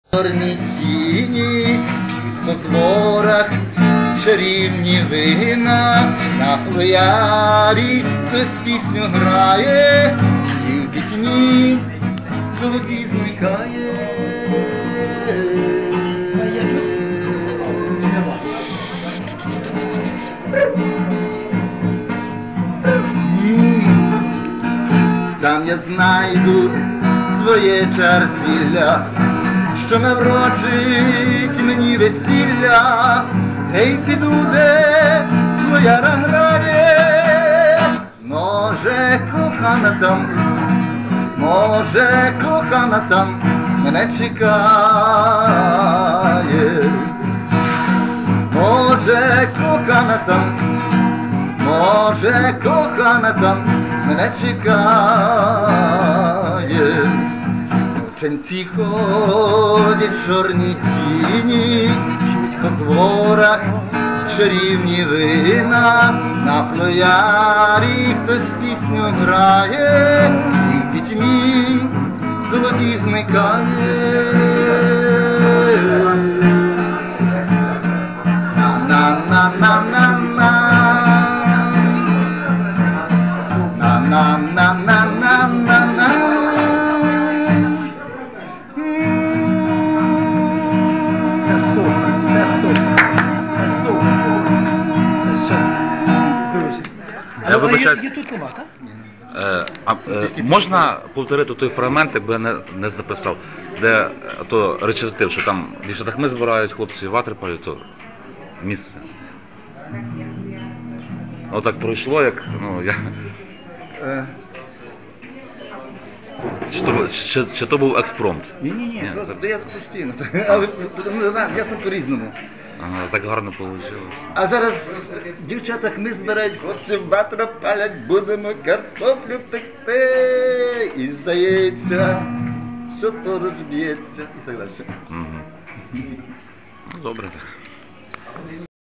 Михайло Дубецький (Львів) у кафе "Соняшник", Верхнє Синьовидне, 2007 р.
Афтерпаті після фестивалю "Бескиди".
Зйомка - Кока Черкаський.

присутні: Кока_Черкаський Маренич_Валєра Скиба_Роман Наталя_Криничанка Михайло_Дубецький та інні